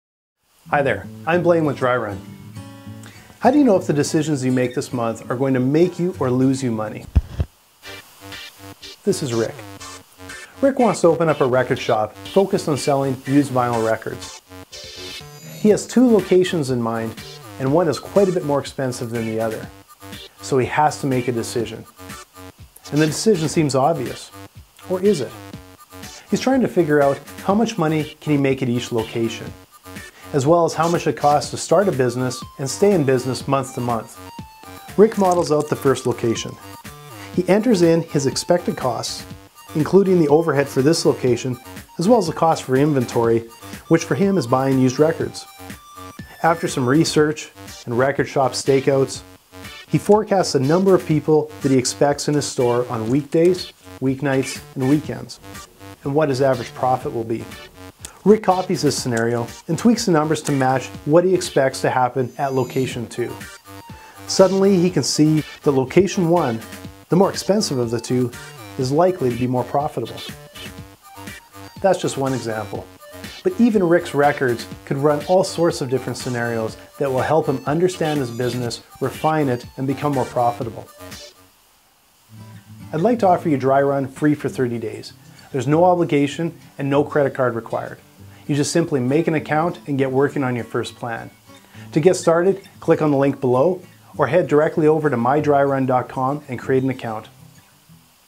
Dryrun : The simple way to picture revenue, profit and cash flow scenarios.
Bring Clarity to Your Numbers: The simple way to picture revenue, profit and cash flow scenarios.

Dryrun makes it simple to picture revenue, profit and cash flow scenarios helping business owners see the impact of their decisions before they happen.

This cloud-based tool is highly flexible, easy-to-use and delivers a crystal clear visualization of key data, but the real power in Dryrun comes through it’s scenario-based approach for evaluating options. From understanding the timing of the money that flows in and out of a business, through to visualizing a sales pipeline and evaluating profit margins, Dryrun helps to uncover the key metrics that mean the most to a business owner.

Dryrun is accessible on nearly any device with a browser and your data is stored securely in the cloud, available when and where you need it.